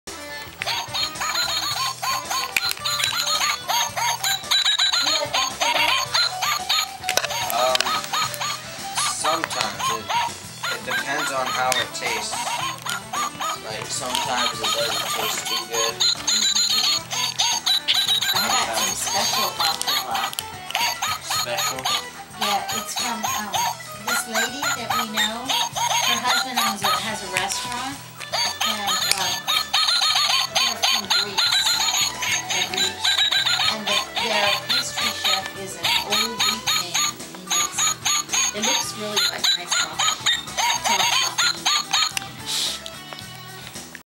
Trudy and Her Musical Christmas Present
Here is our Trudy enjoying her musical reindeer Christmas present.  Sadly, the little reindeer did not survive all the chewing.  Oh well, there's always next Christmas.